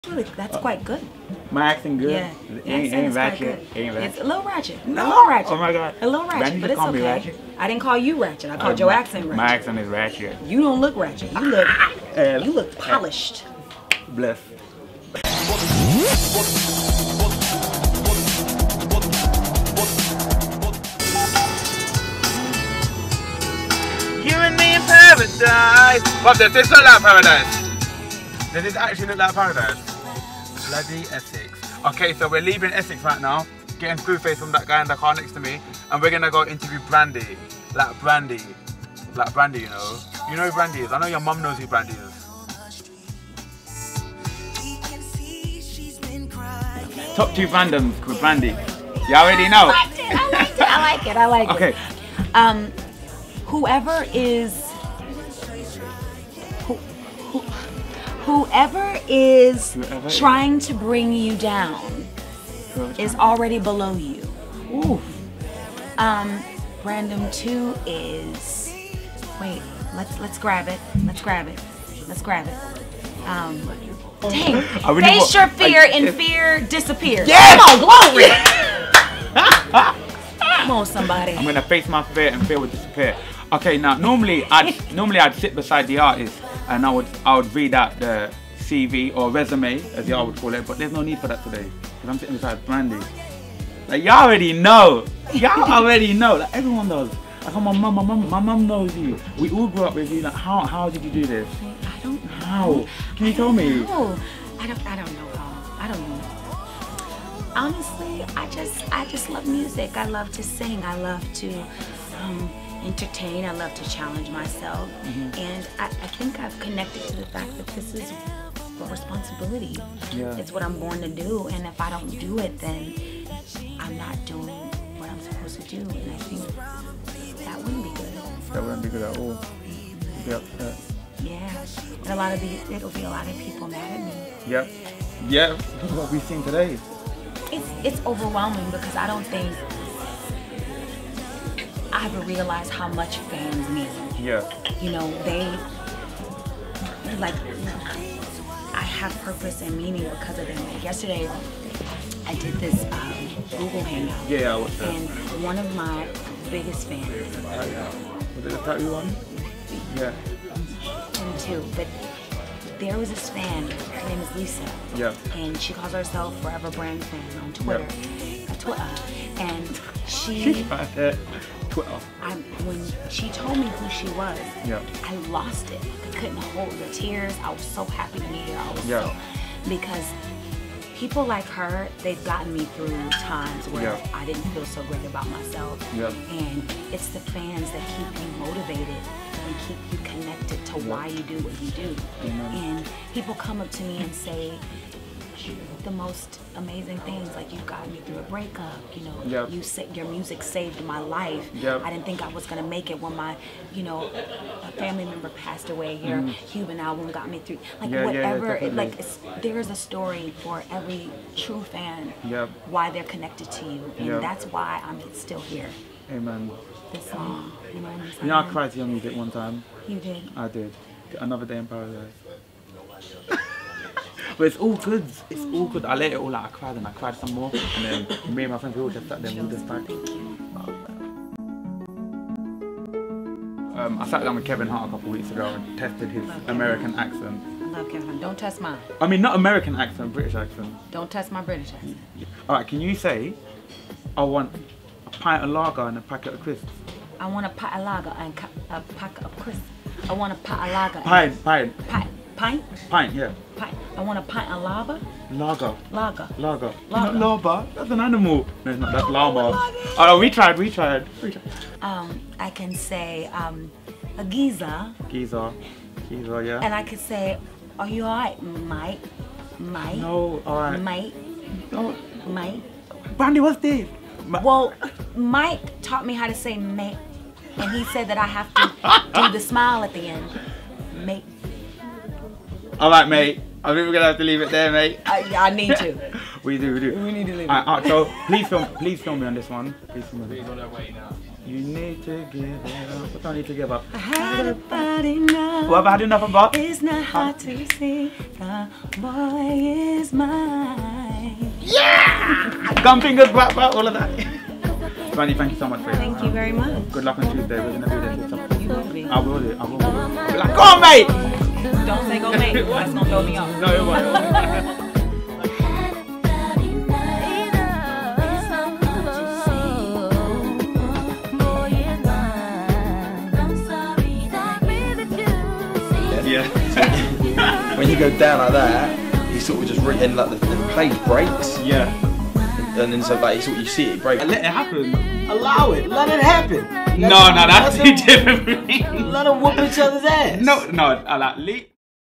Hussain's House: Brandy's Top Brandoms
RNB Legend Brandy Norwood popped in to the House to discuss her headline performance at Musicalize on Tuesday, she tries her UK accent, and has an intimate moment with me when she talks about her fans.